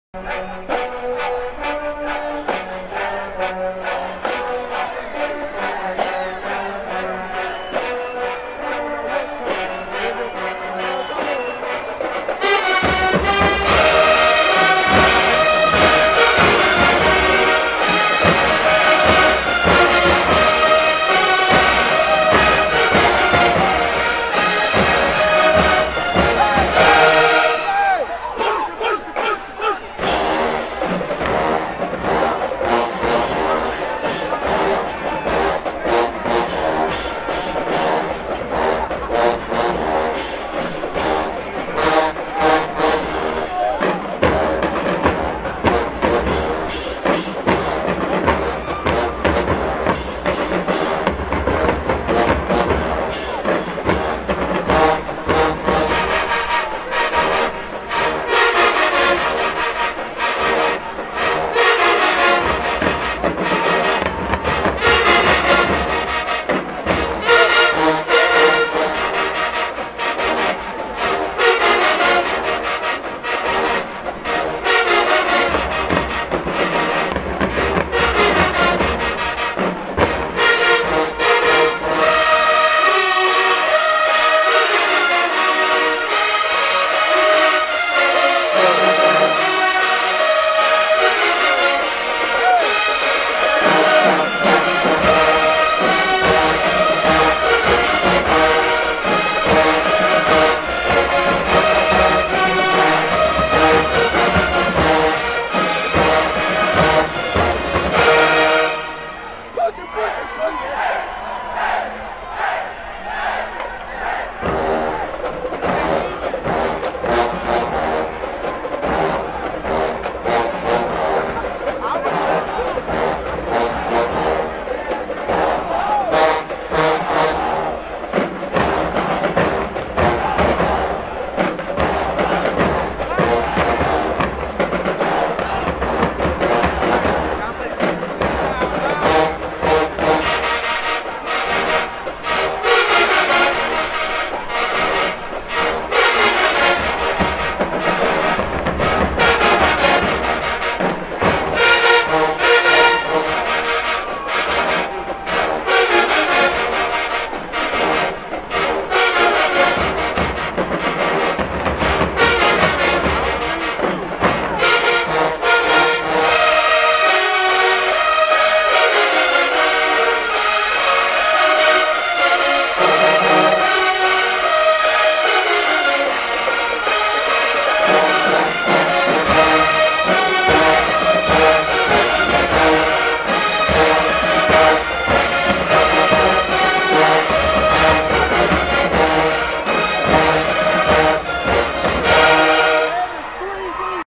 PVAMU Marching Storm (Homecoming 2016)
Homecoming 2016 in the new Panther Stadium.